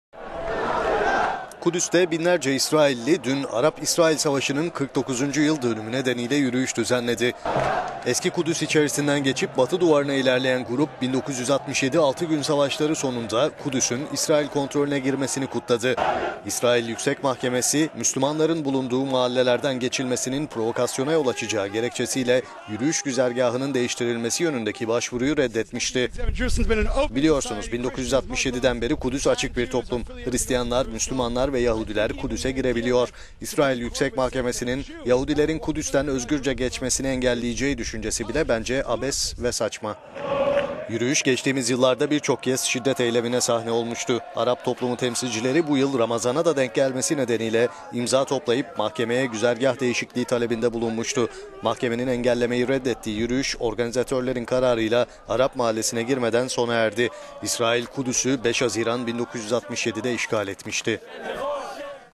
Kudüs'ün işgalini kutladılar
Kudüs'te dün binlerce İsrailli, Arap-İsrail Savaşı'nın 49'uncu yıl dönümü nedeniyle yürüyüş düzenledi.

Tepkilere rağmen Eski Kudüs içerisinden geçip batı duvarına ilerleyen grup, 1967'deki Altı Gün savaşları sonunda Kudüs'ün İsrail kontrolüne girmesini kutladı: "Biliyorsunuz, 1967'den beri Kudüs açık bir toplum. Hristiyanlar, Müslümanlar ve Yahudiler Kudüs'e girebiliyor. İsrail Yüksek Mahkemesi'nin Yahudiler'in Kudüs'ten özgürce geçmesini engelleyeceği düşüncesi bile bence abes ve saçma."